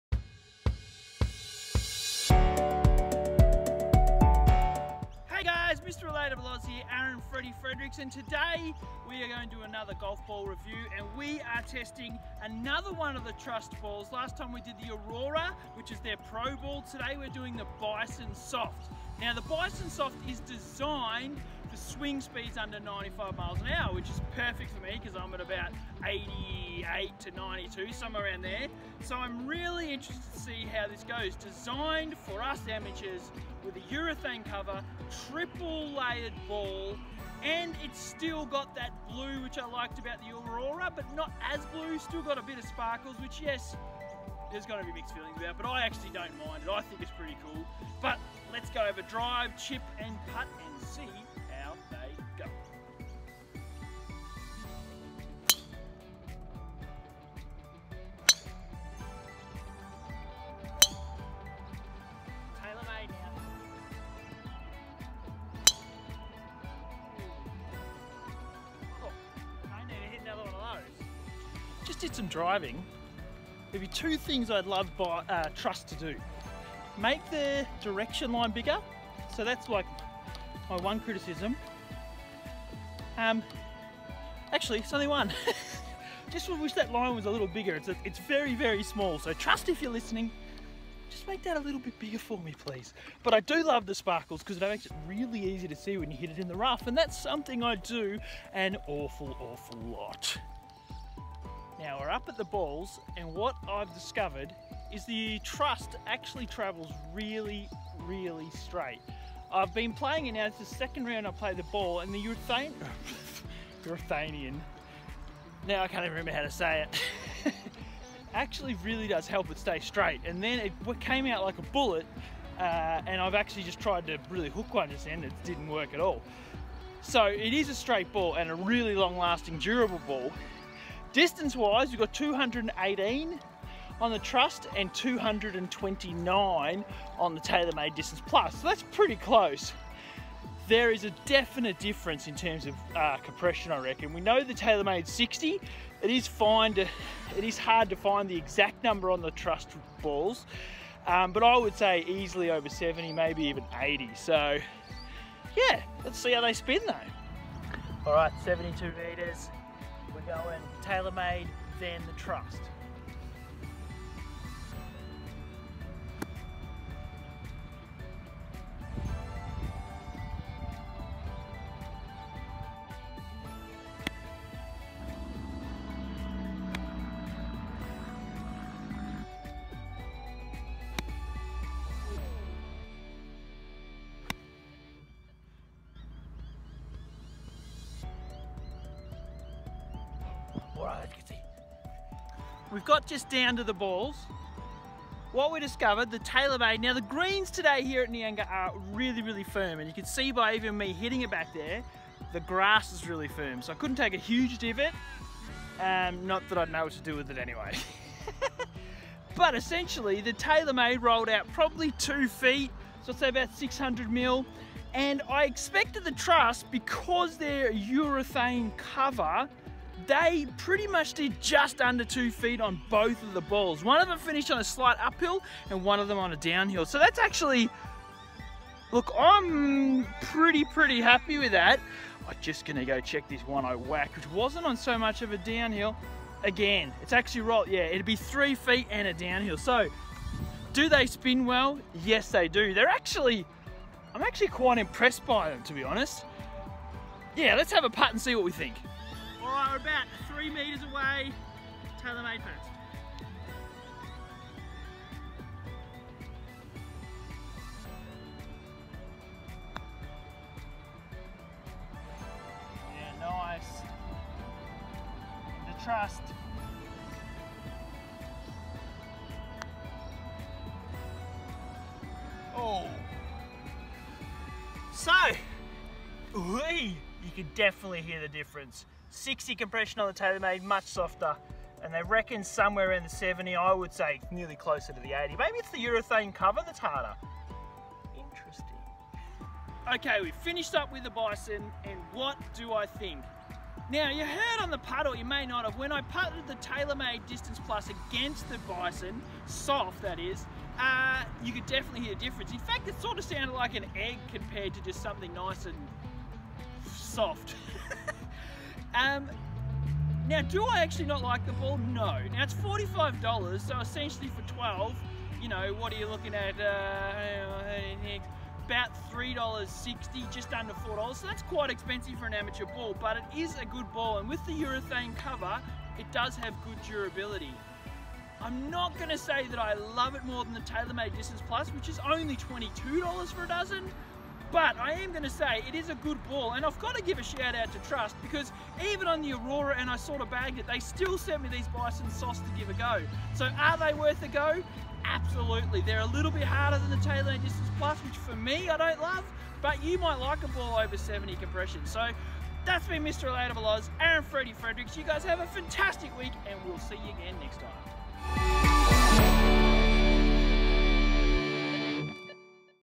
Golf Ball Review:- Trust Bison Soft vs Taylormade Distance+
The Trust Golf Ball is the Softest in their Lineup. It is made for Golfers who have a swing speed under 95mph. Three Layers and a Urethane cover make for a good ball. Have a watch and see.